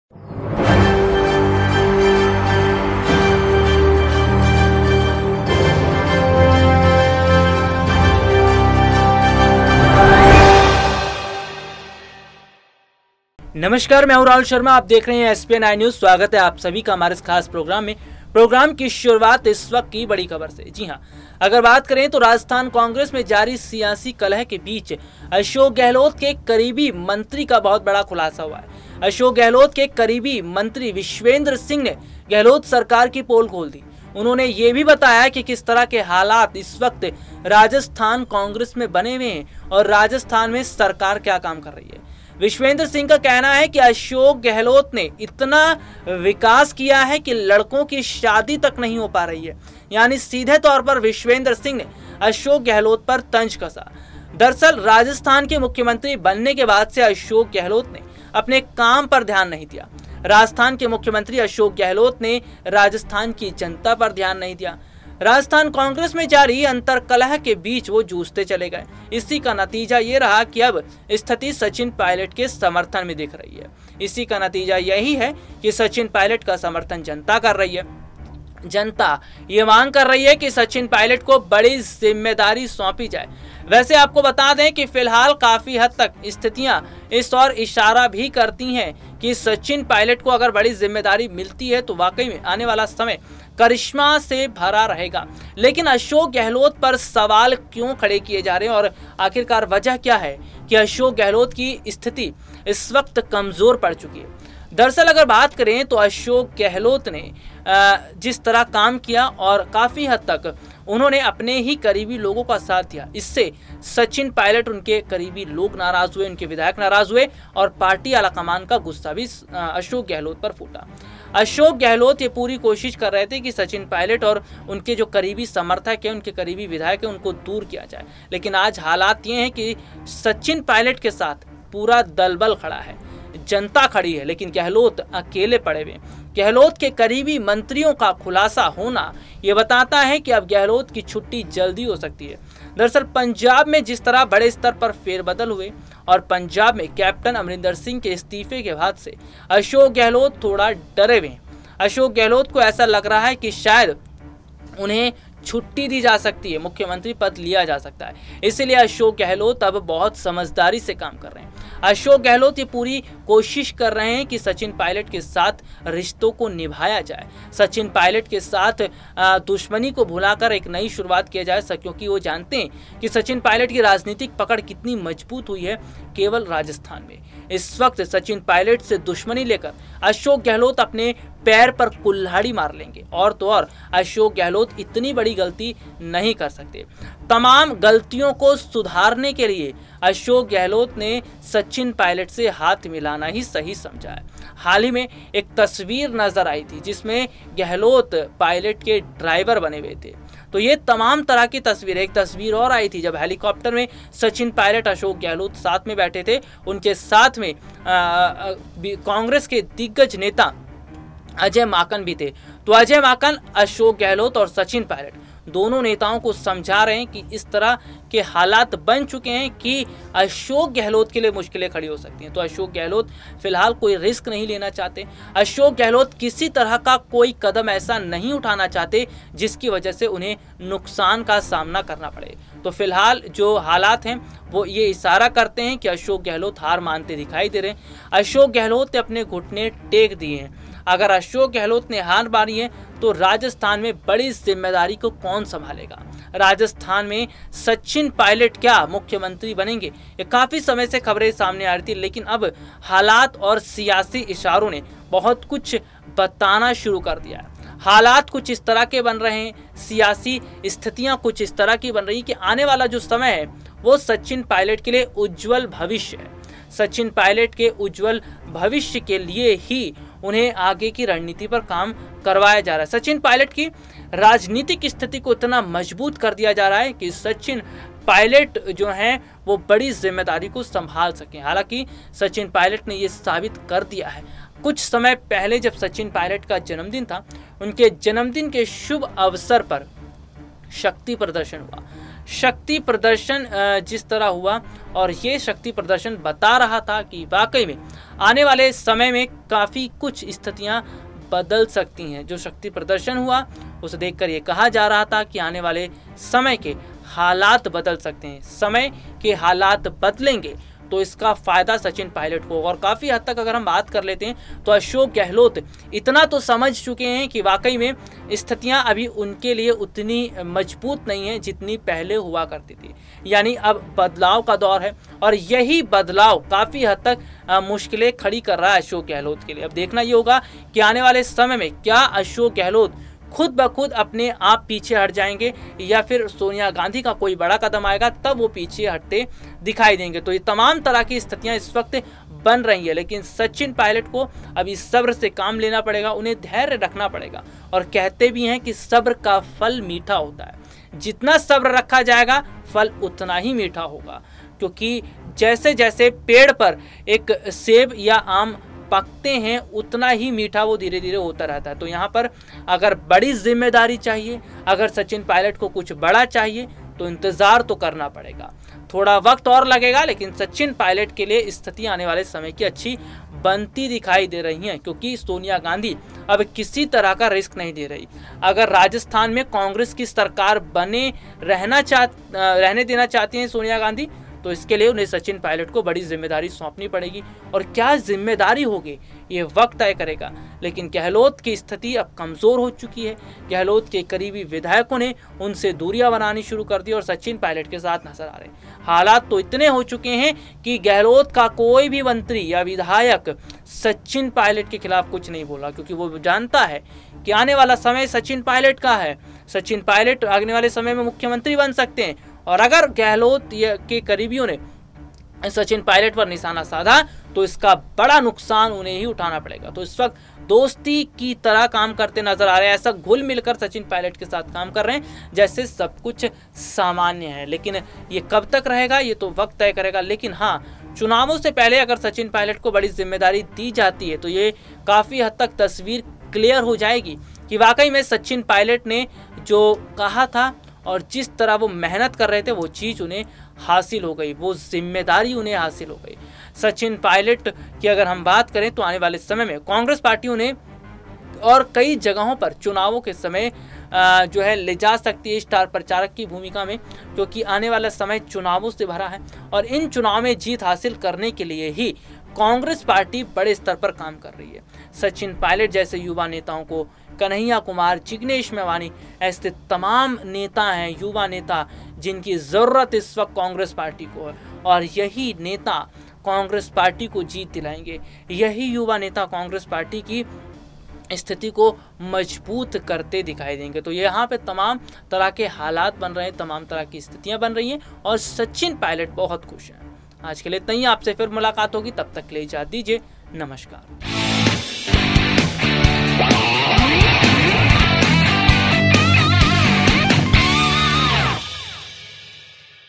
सचिन पायलट के आगे टूट गया गहलोत का घमंड, सोनिया ने दिया बड़ा ऑफर || Sachin Pilot || Congress
सोनिया ने दिया पायलट का साथ, सचिन पायलट के आगे गहलोत ने टेके घुटने, सचिन पायलट को मिलेगी बड़ी जिम्मेदारी